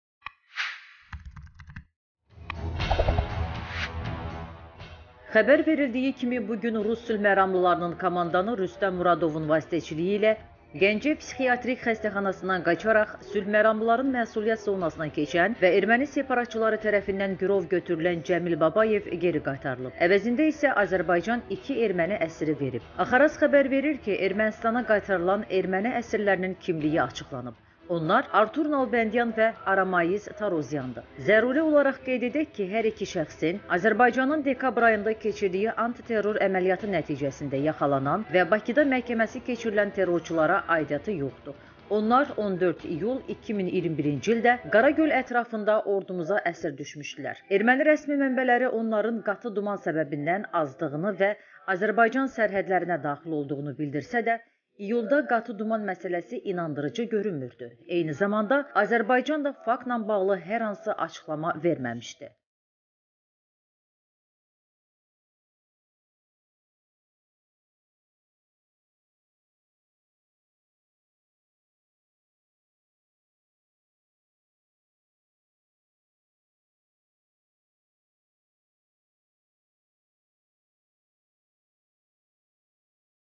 BAKININ VERDİYİ ƏSİRLƏR KİMLƏRDİR? NƏ VAXT ƏSİR DÜŞÜBLƏR?
Xəbər verildiyi kimi, bu gün rus sülhməramlılarının komandanı Rüstəm Muradovun vasitəçiliyi ilə Gəncə psixiatrik xəstəxanasından qaçaraq, sülhməramlıların məsuliyyət zonasına keçən və erməni separatçıları tərəfindən girov götürülən Cəmil Babayev geri qaytarılıb, əvəzində isə Azərbaycan iki erməni əsiri verib.